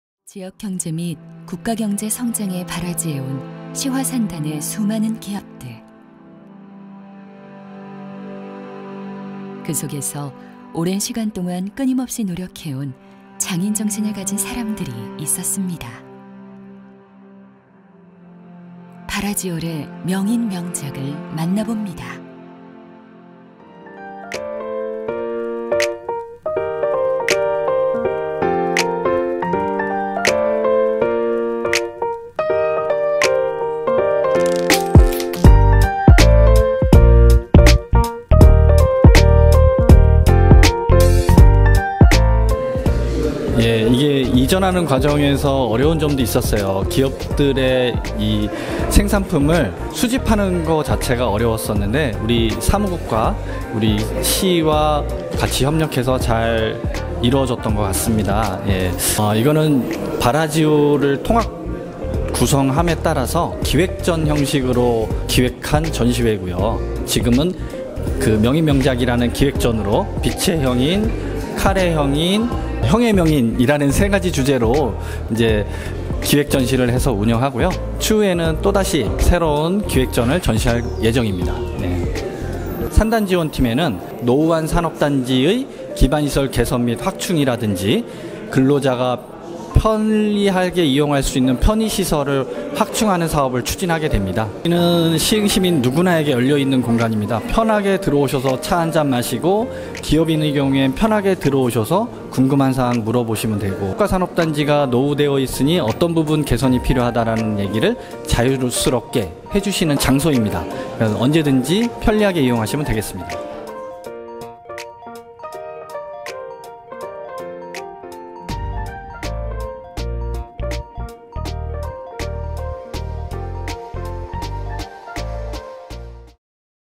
이용하시흥 상설전시장
시흥 기업체생산품 전시장 및
산단지원팀이 새롭게 사무실을 열었습니다.

최고의 제품 생산을 위해
끊임없이 노력하는 장인들의 작품도 만나보시흥!~

📍시화국가산업단지 지원업무 수행

📍기획전시 : 명인명작(名人名作) 기획전
📍장소 : 시흥비즈니스센터 1층